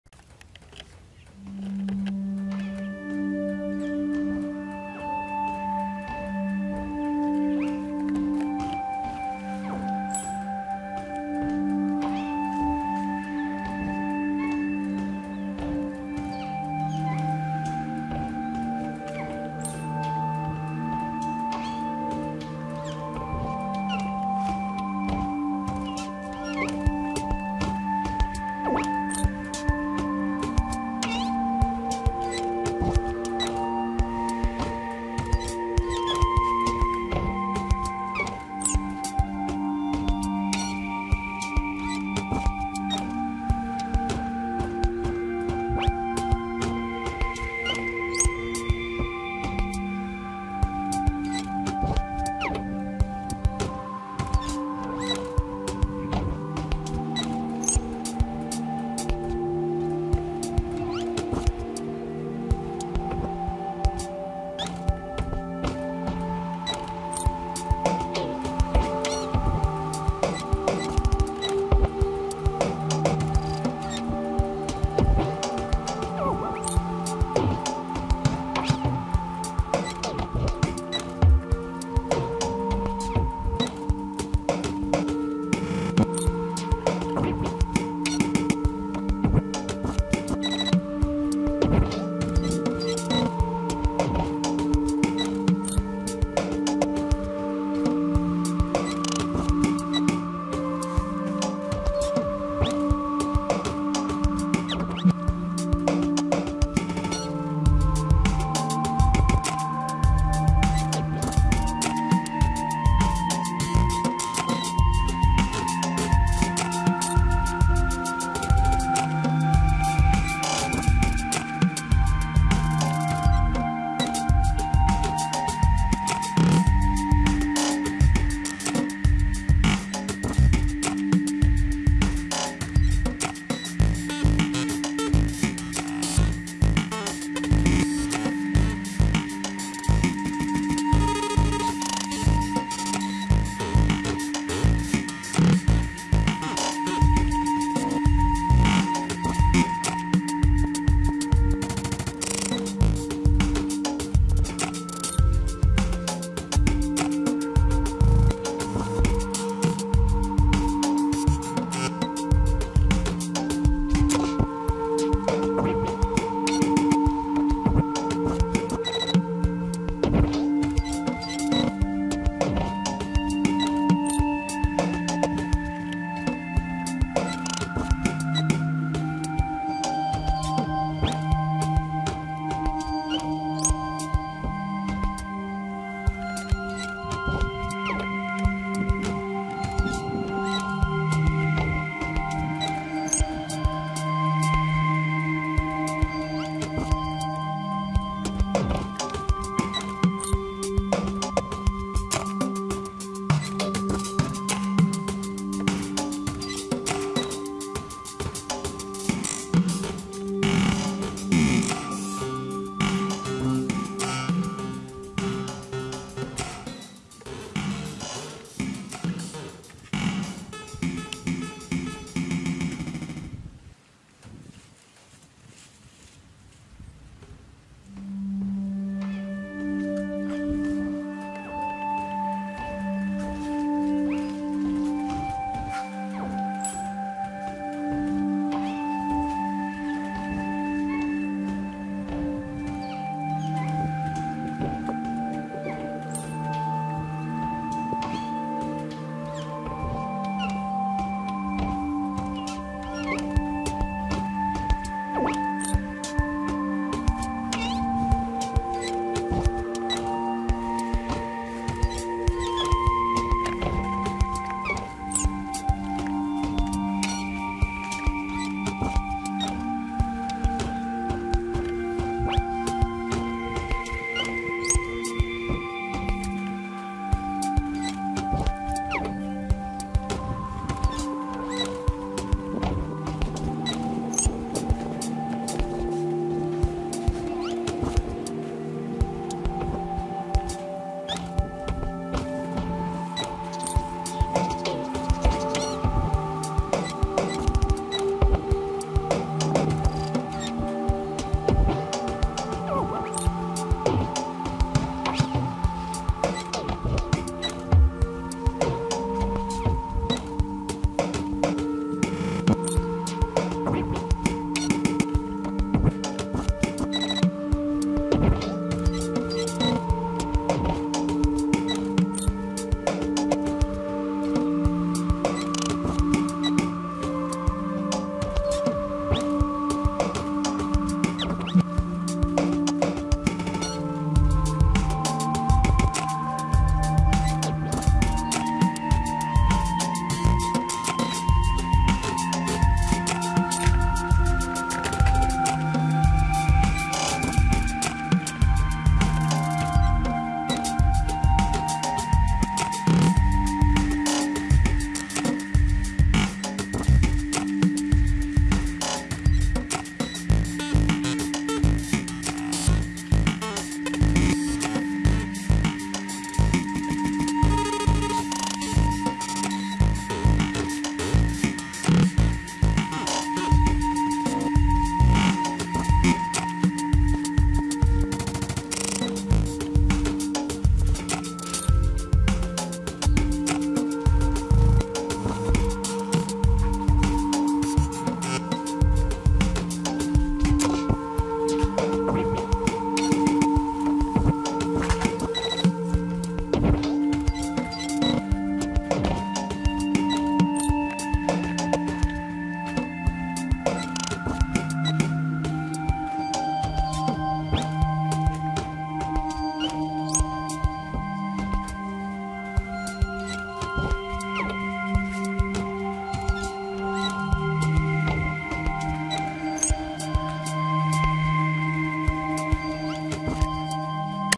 Buffaloes Group Walking in South Land Lot Eat Grass New Red Road/Go To Stable in Svay Thom Village
Buffalo Group Walking in South Land Lot Eat Grass Cross New Red Road/Go To Stable in Svay Thom Village